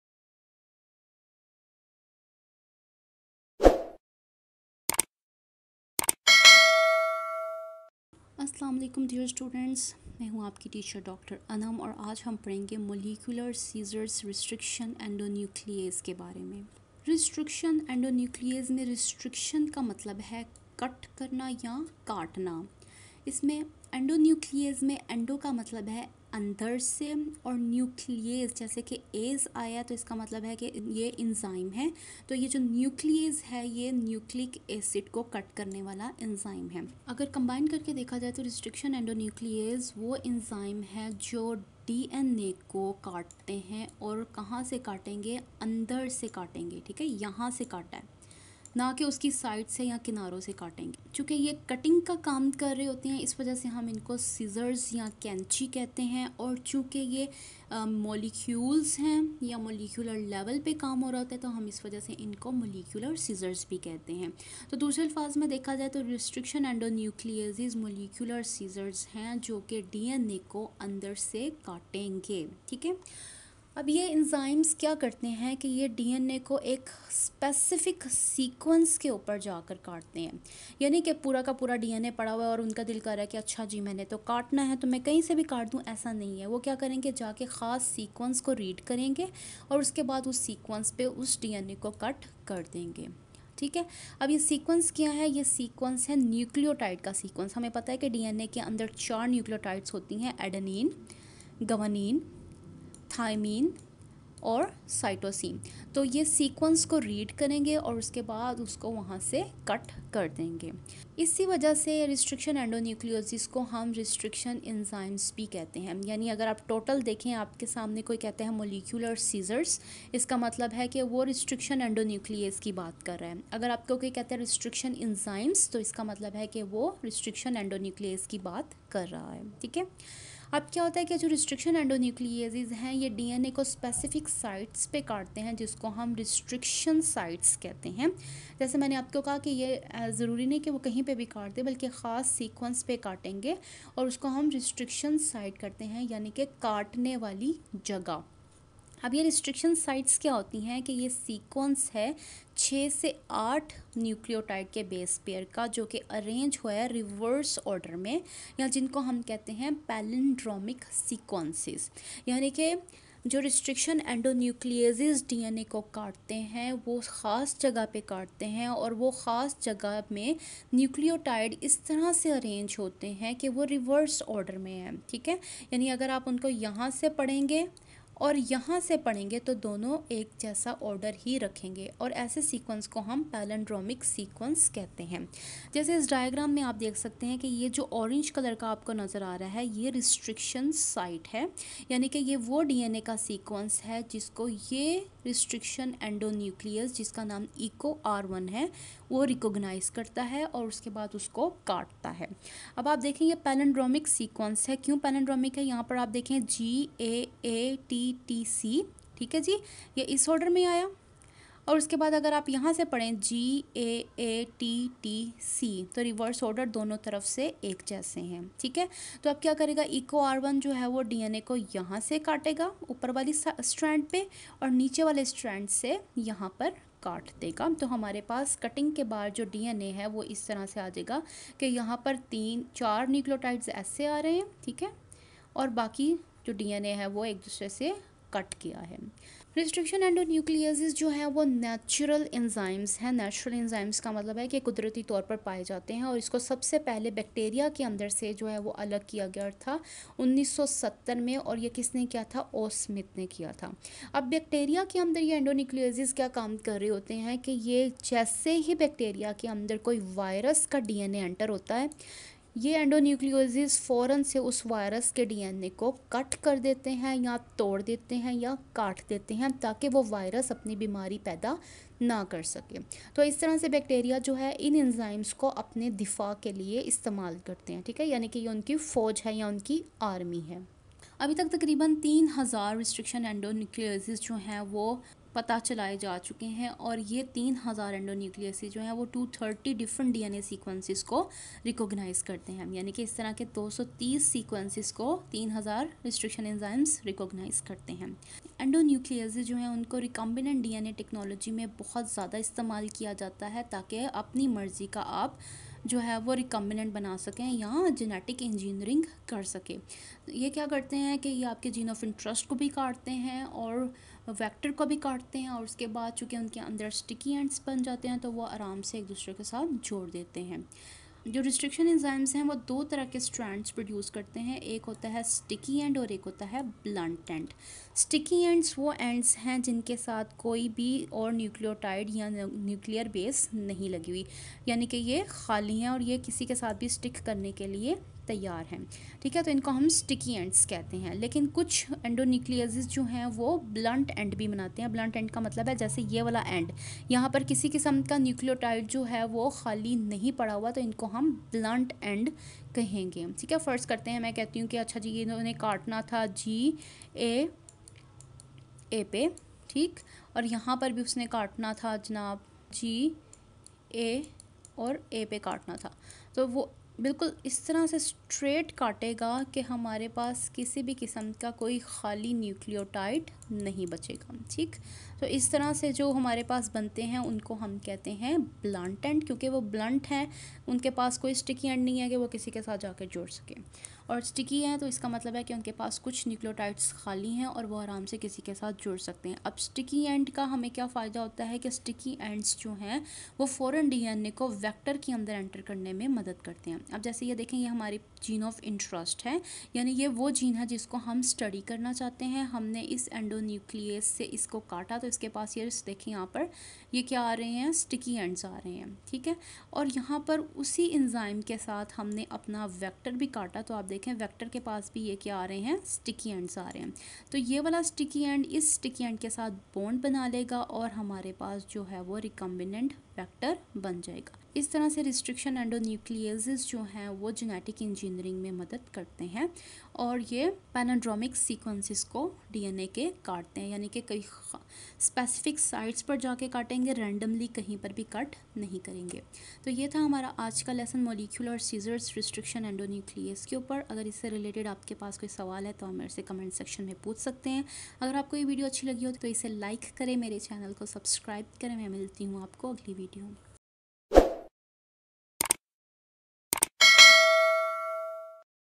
Molecular Scissors Restriction Endonuclease in Urdu/Hindi | Biotechnology
Molecular Scissors Restriction Endonucleases are enzymes that recognize a specific DNA sequence, called a restriction site, and cleave the DNA within or adjacent to that site. They are also known as restriction enzymes.